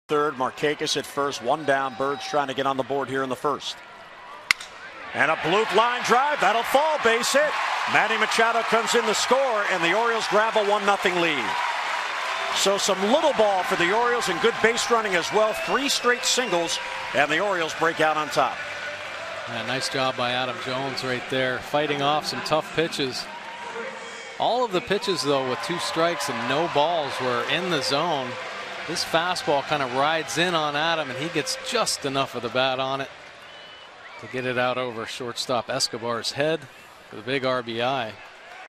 2013/05/07 Jones' RBI single
KC@BAL: Jones' RBI single puts the O's up early